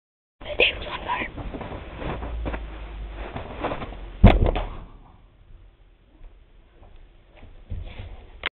wow
my crazy little brother jumping around thinking hes a wrestler!